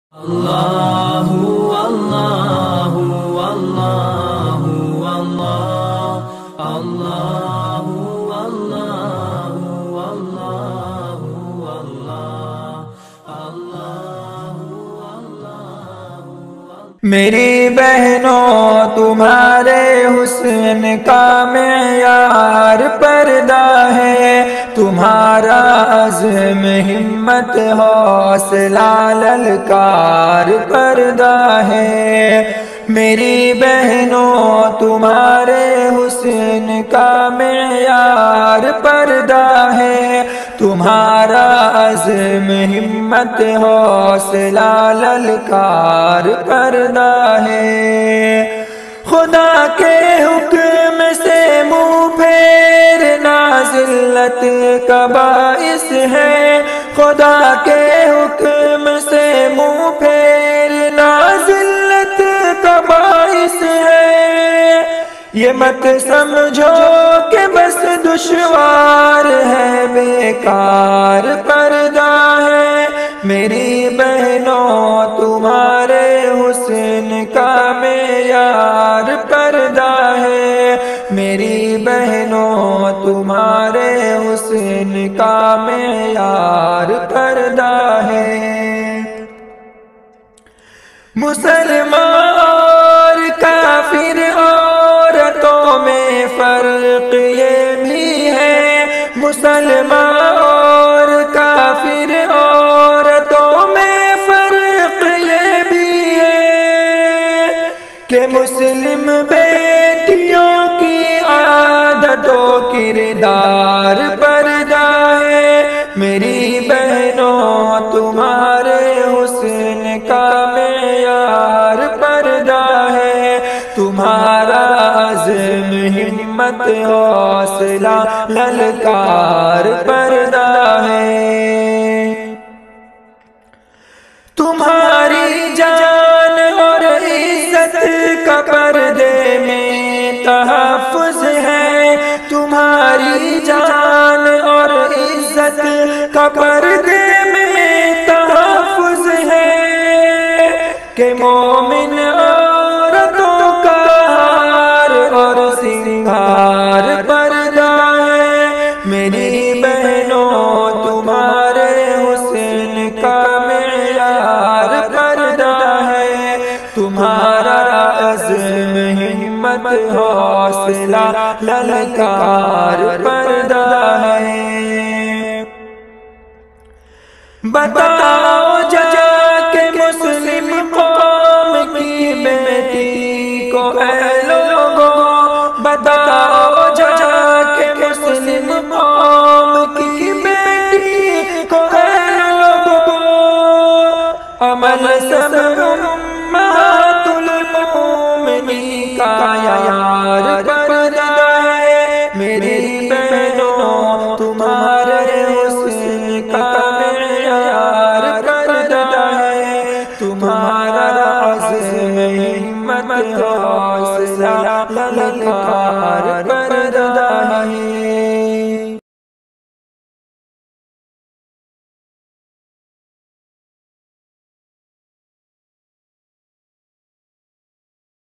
Heart touching poetry for women. hijab nazam 2021 vocalist Mushtaque ahmad
Hijab nazam. New poetry.      2021

Video.editing Mushtaque ahmad
Video recording. Mushtaque ahmad
Upload by. Mushtaque ahmad official
Special thanks. Hafiz Dilshad Sahab